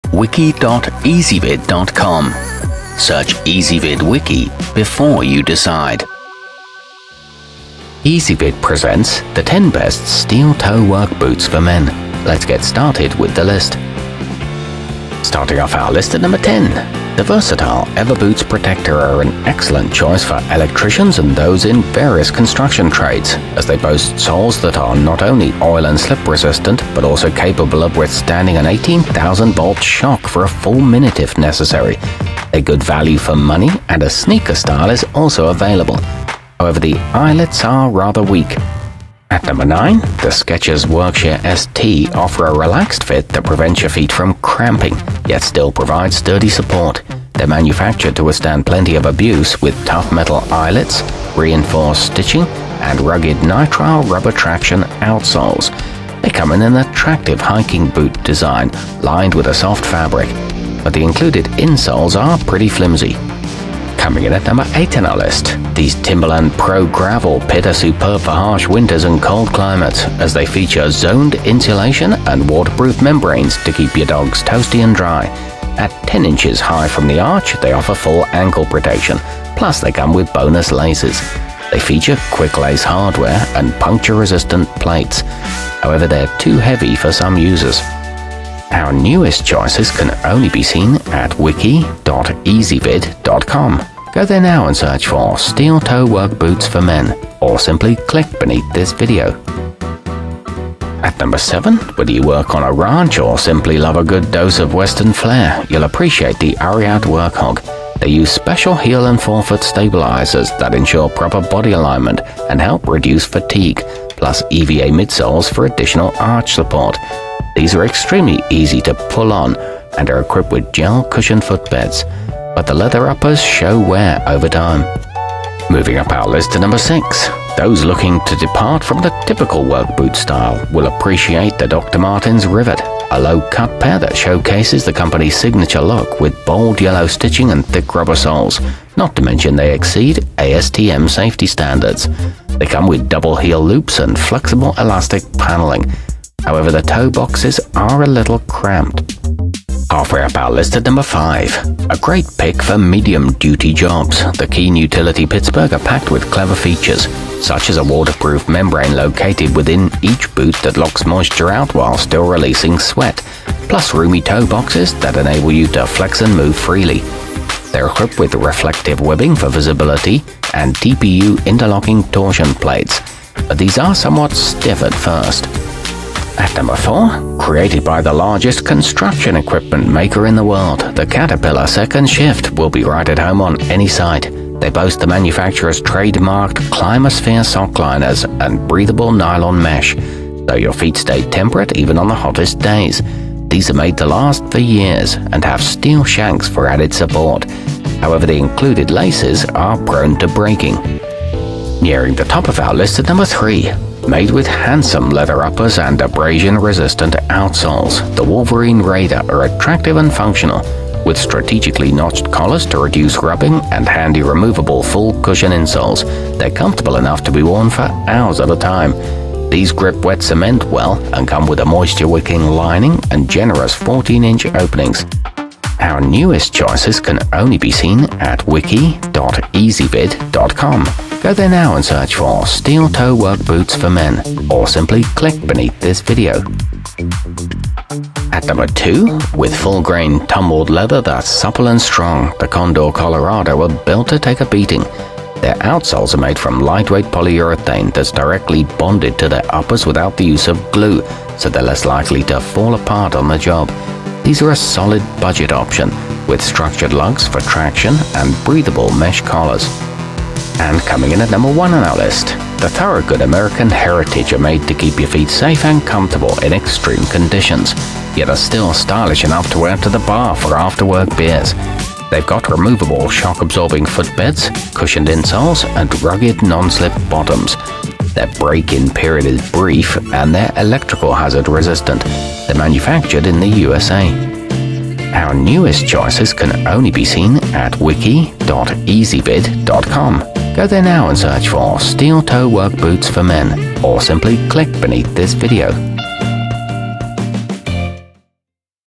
10 Best Steel Toe Work Boots For Men 2018
Our complete review, including our selection for the year's best steel toe work boot for men, is exclusively available on Ezvid Wiki.

Steel toe work boots for men included in this wiki include the thorogood american heritage, ever boots protector, dr. martens rivet, skechers workshire st, caterpillar 2nd shift, condor colorado, ariat workhog, keen utility pittsburgh, wolverine raider, and timberland pro gravel pit.